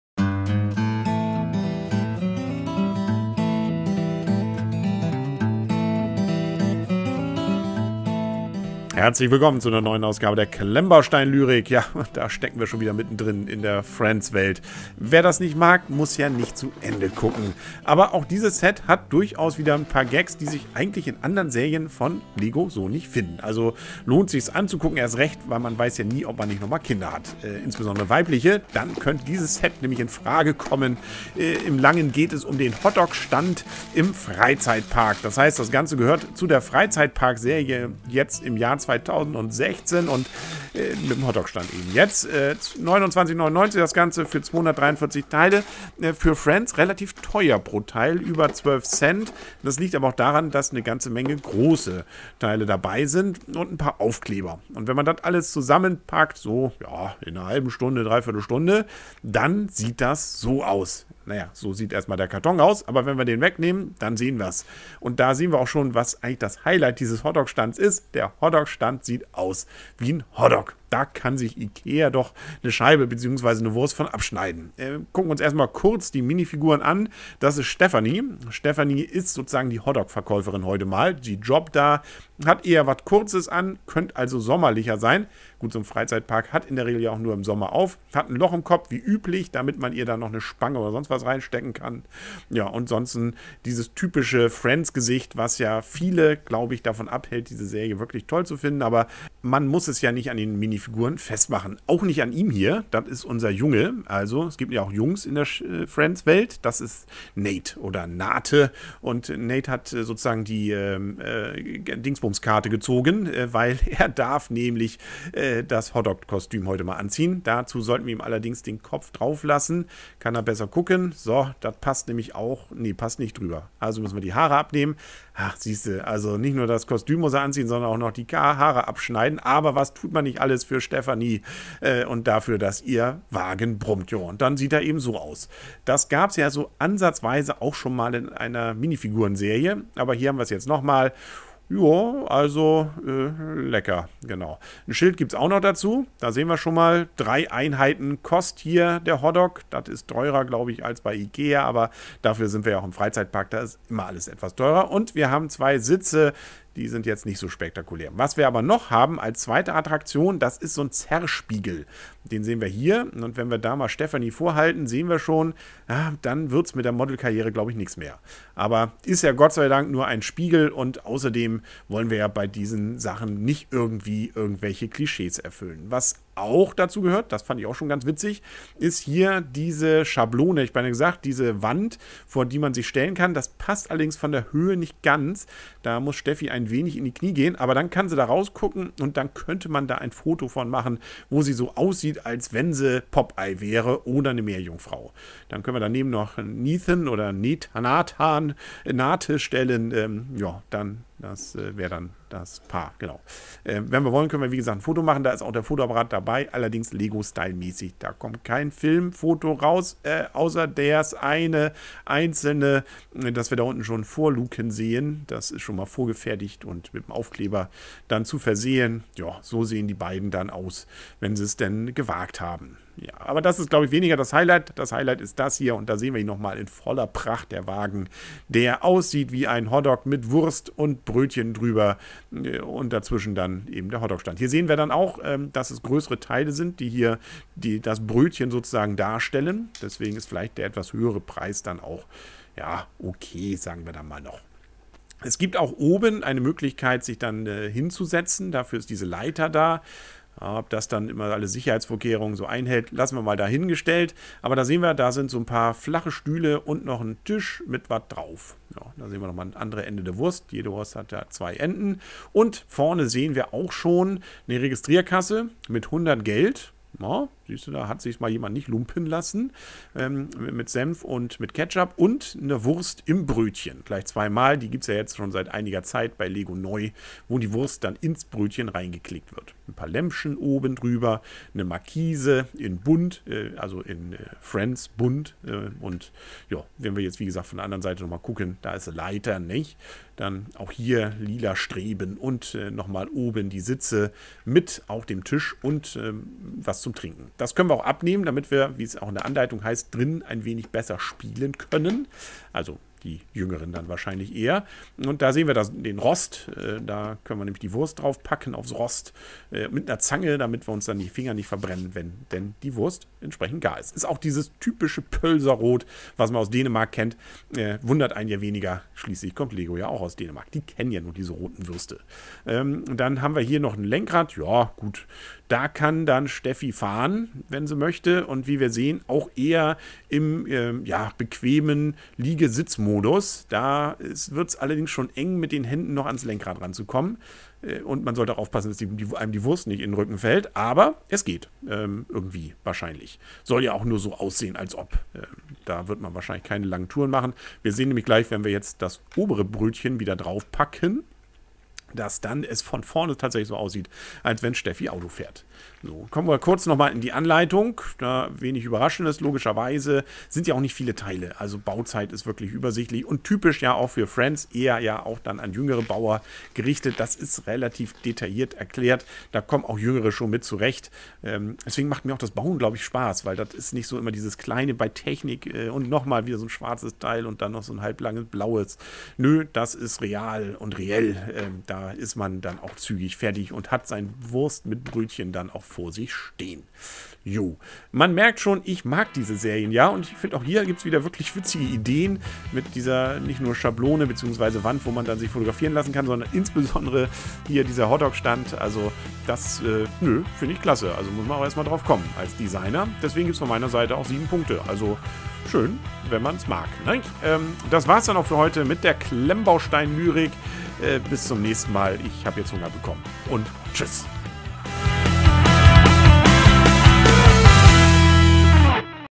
LEGO Hot-Dog-Stand im Freizeitpark (Set 41129 Friends Review deutsch 4K)
Jeder gute Freizeitpark braucht auch einen Hot-Dog-Stand. Das müssen sich auch die Designer bei LEGO-Friends gedacht haben, denn der 2016er Serie des Friends-Freizeitparks hat man selbigen Imbisswagen spendiert. Und damit das verkaufte Produkt auch jeder von Weitem erkennen kann, ist gleich der ganze Stand wie ein Hot-Dog gestaltet. Mit dabei sind zwei Minifiguren (Stephanie und Nate).

Das Set über folgenden Link bei Amazon kaufen und durch die Provision die Klemmbausteinlyrik etwa unterstützen